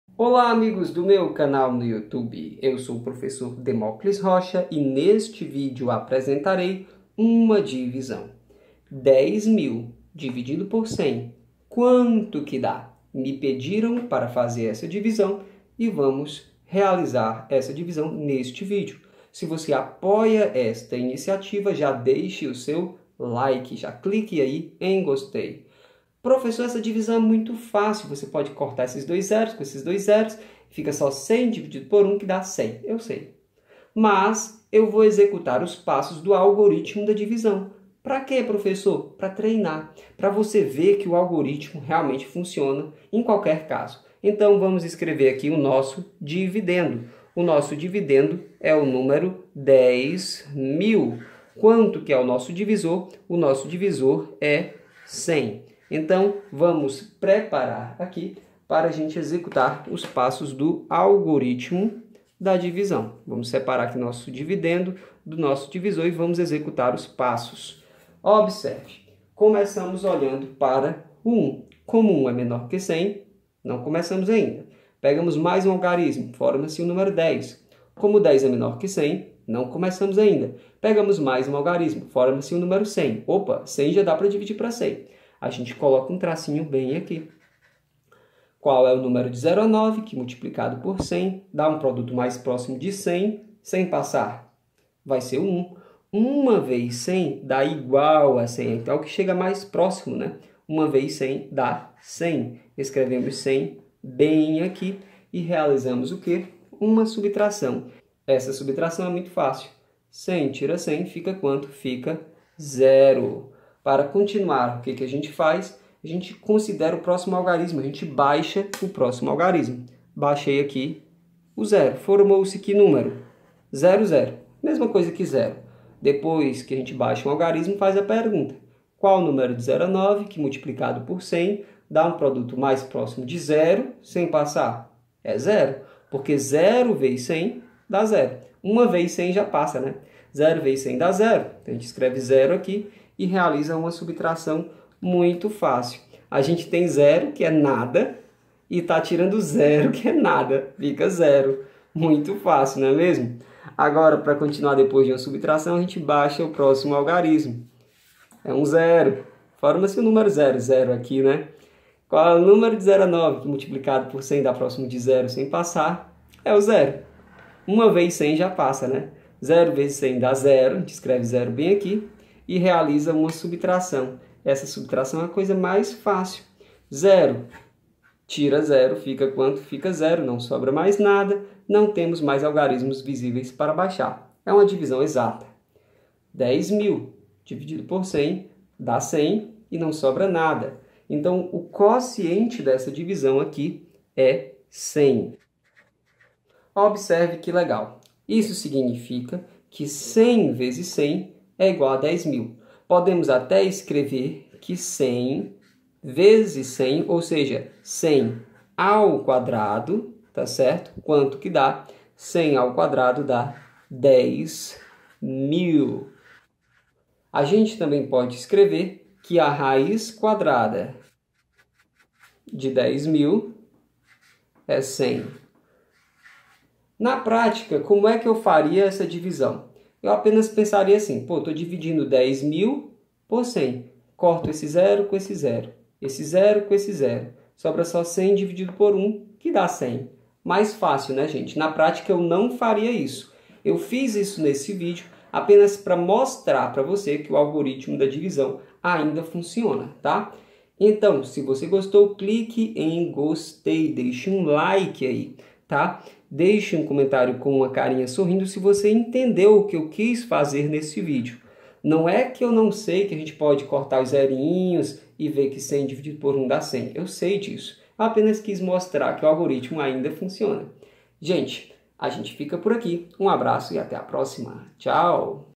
DIVISÃO EXATA: 10000 dividido por 100
Neste vídeo, apresento o quociente da divisão de 10000 por 100. Mesmo sabendo que podemos simplificar e obter rapidamente o resultado, que é 100, eu executo os passos do algoritmo para treinar e mostrar que ele ainda é válido mesmo nessa situação.
Como o quociente é 100 e o resto é 0, concluo que 10000 é o mesmo que 100*100. Assim, 100²=10000 e também a raiz quadrada de 10000 é 100. Espero que gostem. Bom estudo!

Sony Vegas

Atube Catcher

MICROFONES

Lapela Sony: ECMCS3